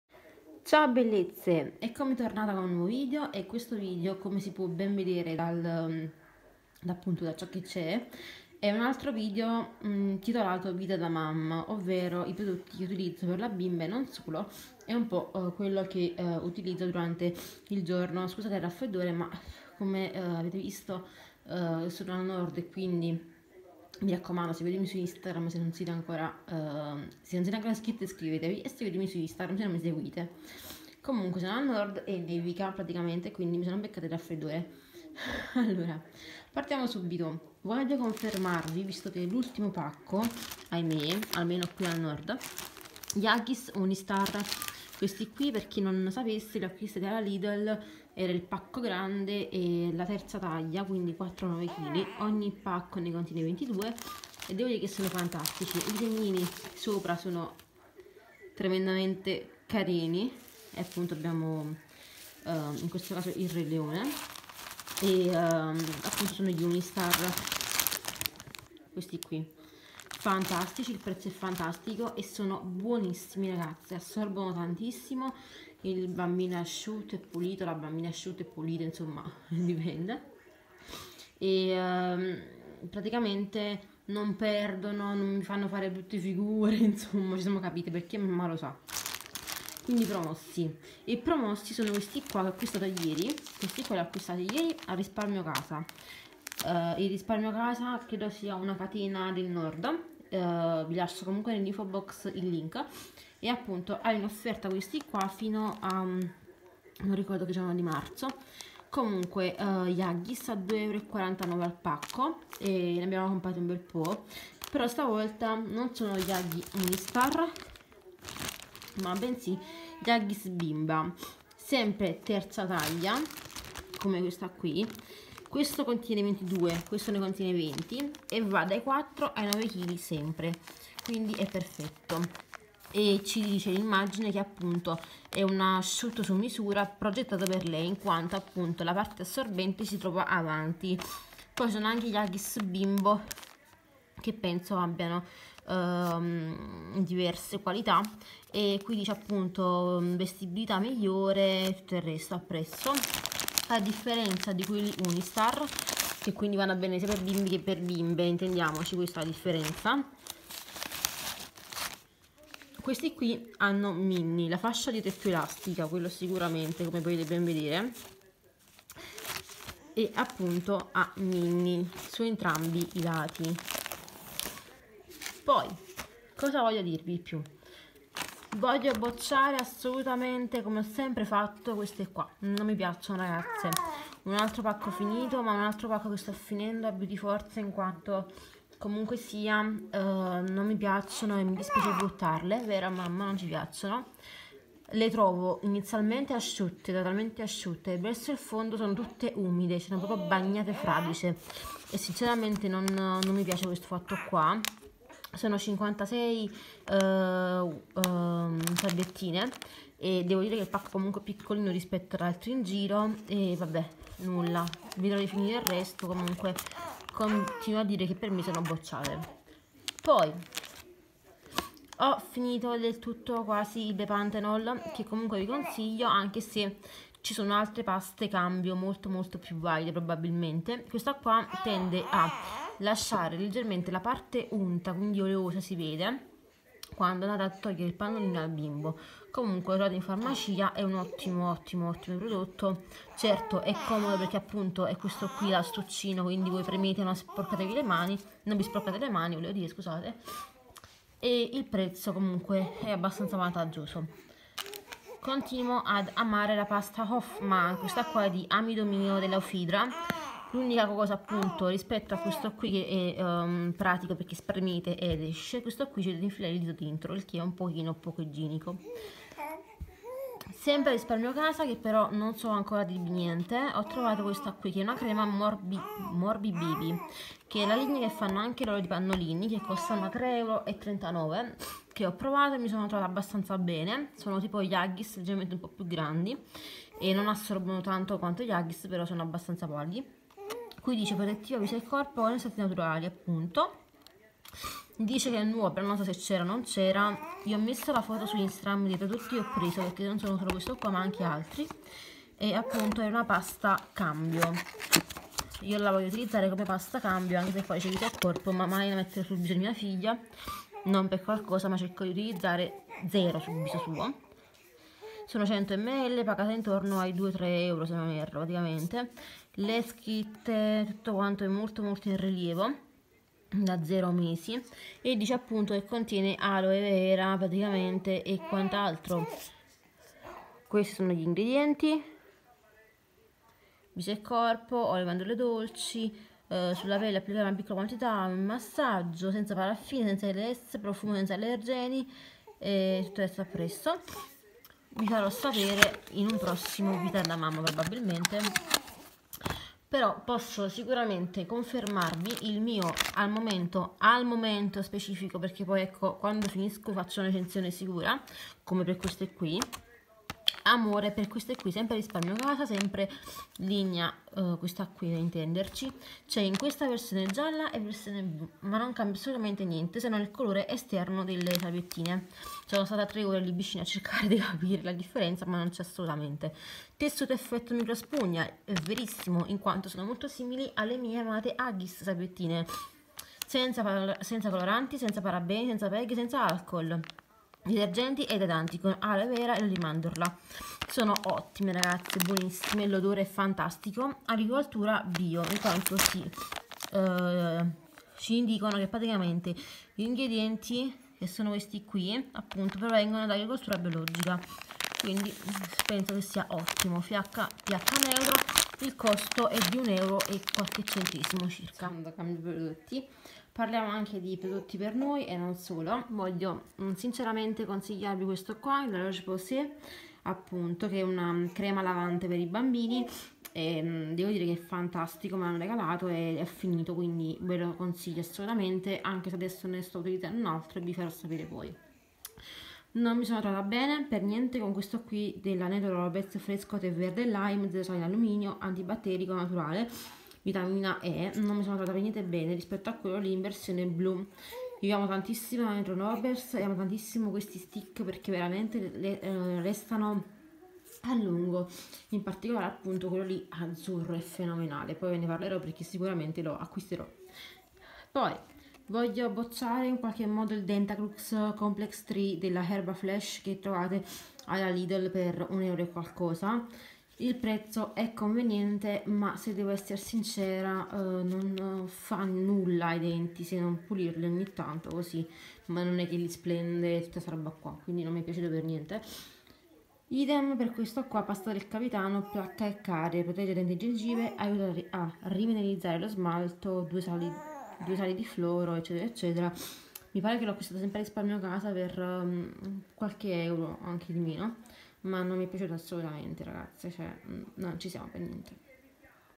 Vita da mamma #3!
INFORMAZIONI UTILI

Ciao bellezze!

Vi mostro un po i prodotti che sto usando e vi dico il mio pensiero!
Sia per quanto riguarda la cura della mamma e sia per quanto riguarda la cura del bebé

link diretti ai vari tutorial che ho girato sull'agenda!

istagram : laragazzaconilcodino

I PRODOTTI MOSTRATI NEL VIDEO SONO STATI INTERAMENTE ACQUISTATI DA ME.